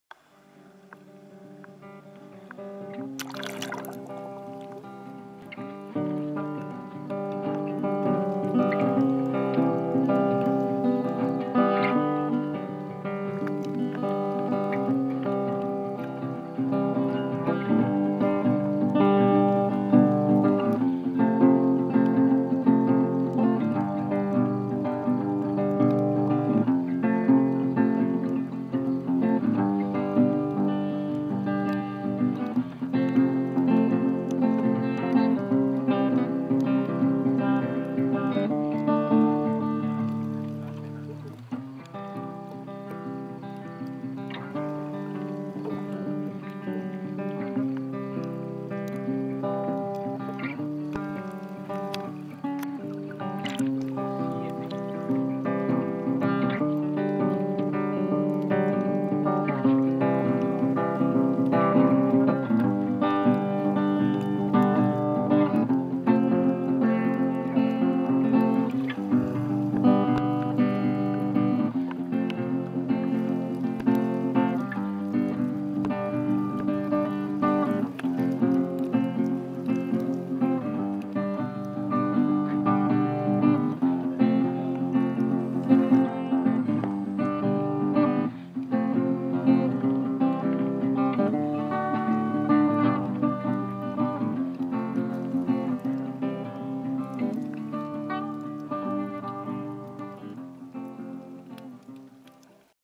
Exciting snorkelling on Crete
Chersonissos; crushing a sea urchin, many fish species